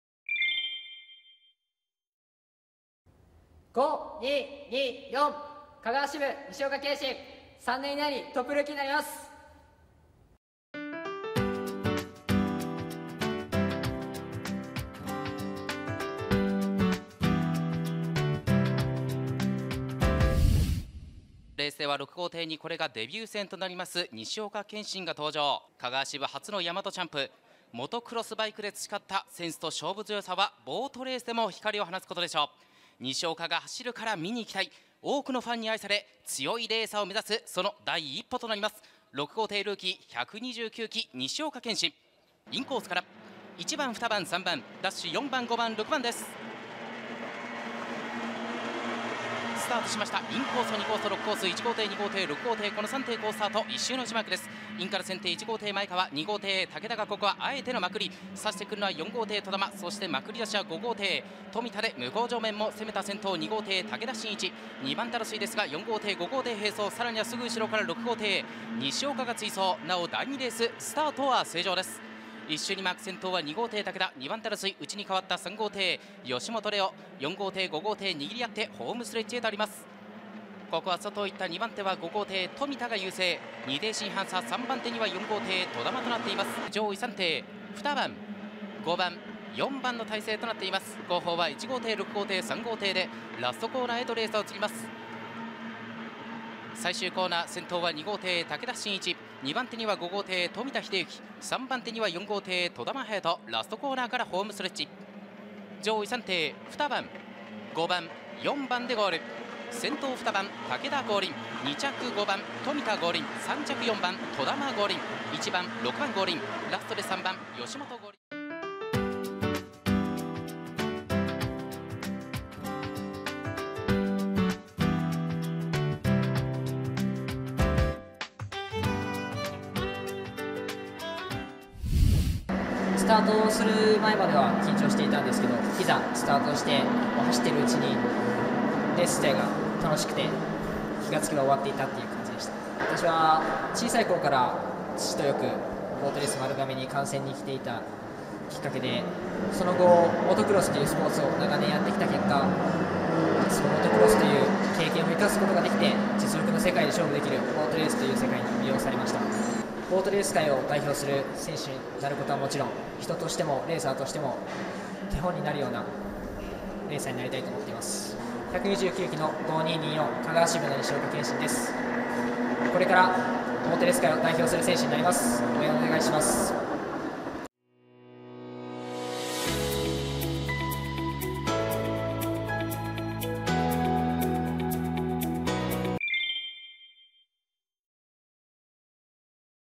BOATCAST NEWS│129期チャンプ 西岡顕心デビュー戦 大きな夢に向かい発進！ ボートレースニュース 2021年11月5日│
西岡顕心選手のデビュー戦！丸亀 一般 初日2Rをご覧ください。

最新のボートレースニュースをお届けします！